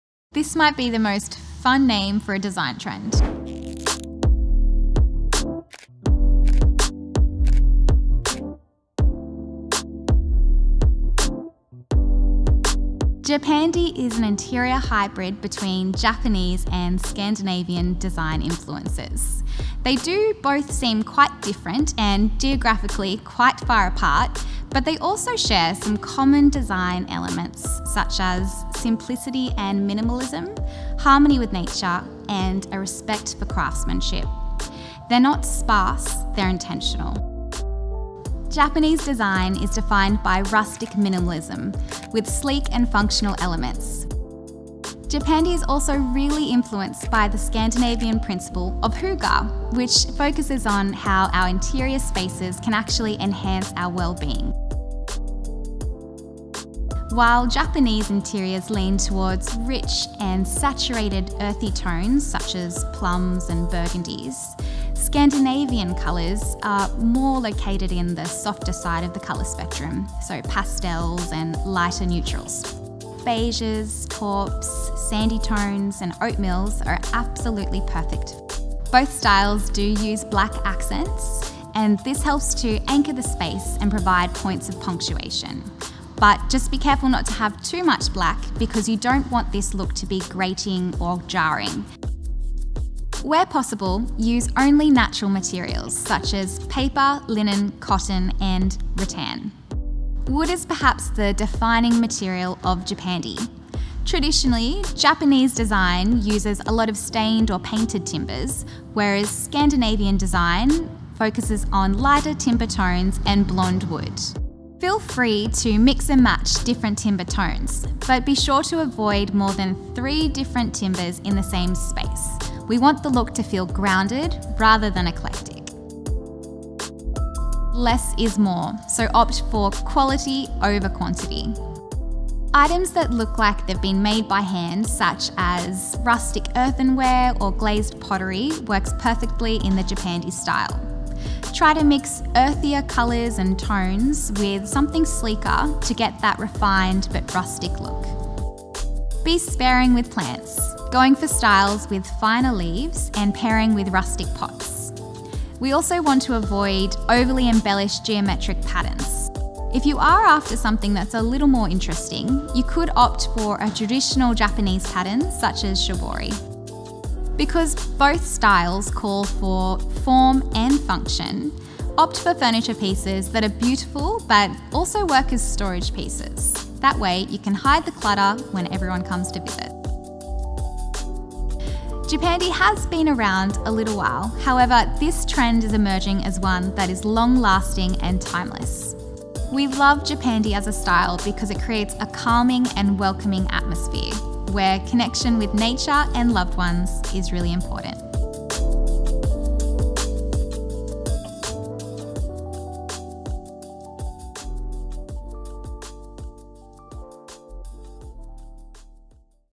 Style Insider by Temple & Webster | What is Japandi?
Confused about what exactly Japandi is? We got you 😎 In our new series Style Insider, Stylist Allira talks origins, colours, shapes and textures so you can bring a little Japandi into your world. 🍵